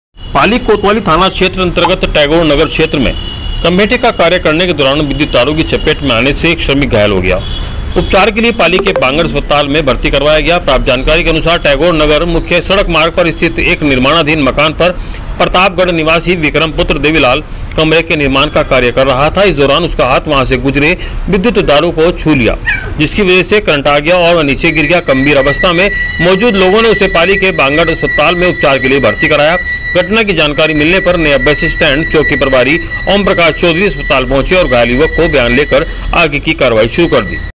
भवन का निर्माण कार्य करते समय बिजली के तारों के चपेट में आने से झुलसा,श्रमिक
उपचार के लिए लाया बांगड़ अस्पताल,,,,
मामले की जानकारी लेने पहुंचे नया बस स्टैंड चौकी प्रभारी,,,,

पाली के टैगोर नगर भवन निर्माण के दौरान हुआ हादसा,,,,

पाली,,

कोतवाली थाना क्षेत्र अंतर्गत टैगोर नगर क्षेत्र में कमठे का कार्य करने के दौरान विद्युत तारों की चपेट में आने से एक श्रमिक घायल हो गया। जिसे उपचार के लिए पाली के बांगड़ अस्पताल में भर्ती करवाया गया। प्राप्त जानकारी के अनुसार टैगोर नगर मुख्य सड़क मार्ग पर स्थित एक निर्माणाधीन मकान पर प्रतापगढ़ निवासी विक्रम पुत्र देवीलाल कमरे के  निर्माण  का कार्य कर रहा था इस दौरान उसका हाथ वहां से गुजरे विद्युत तारों को छू लिये, जिसकी वजह से उसे करंट आ गया और वह नीचे गिर गया गंभीर अवस्था में वह मौजूद लोगों ने उसे पाली के बांगड़ अस्पताल में उपचार के लिए भर्ती कराया घटना की जानकारी मिलने पर नया बस स्टैंड चौकी प्रभारी ओमप्रकाश चौधरी  अस्पताल पहुंच  घायल युवक का बयान लेकर आगे की कार्यवाही शुरू की।